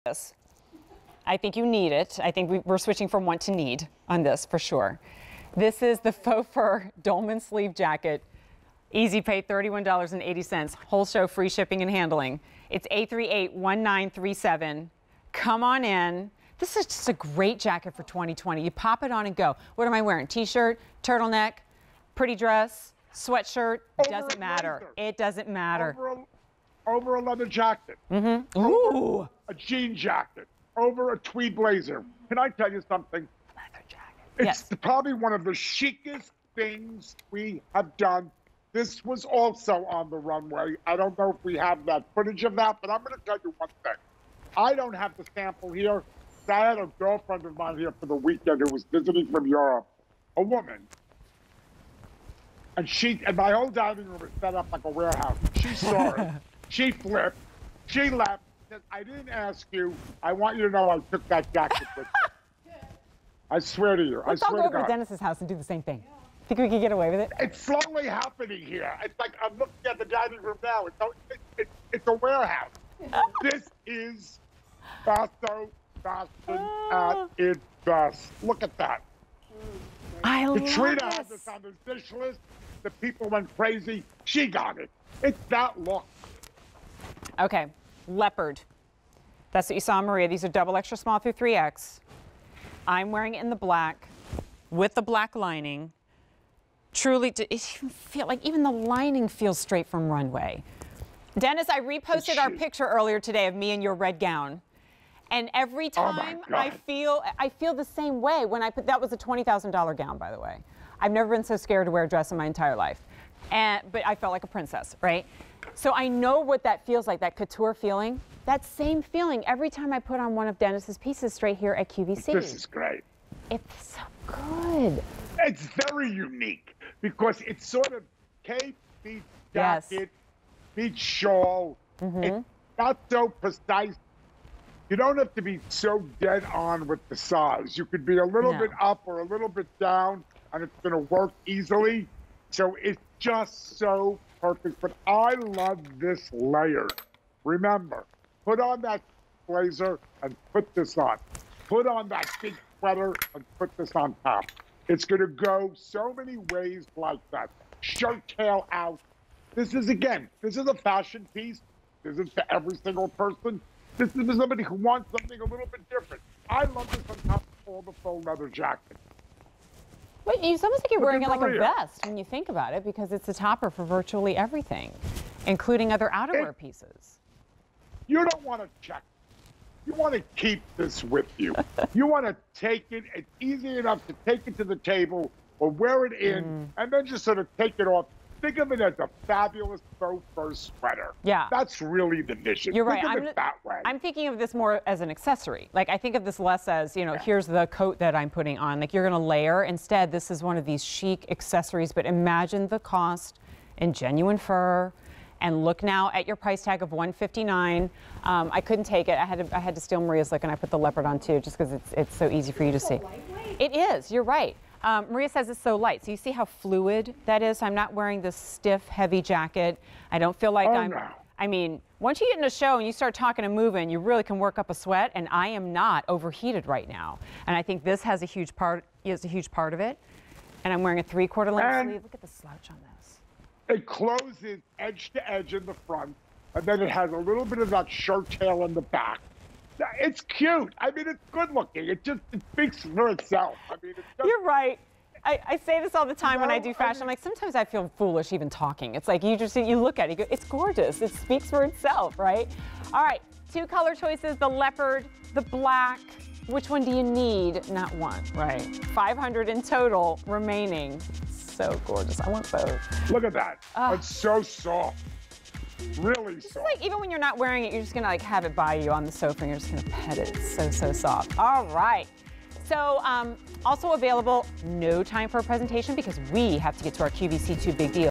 Dennis Basso Faux Fur Dolman Sleeve Jacket on QVC
Dennis Basso Faux Fur Dolman Sleeve Jacket

This dolman-sleeve faux fur jacket will add a luxe touch to your everyday ensembles. From Dennis Basso.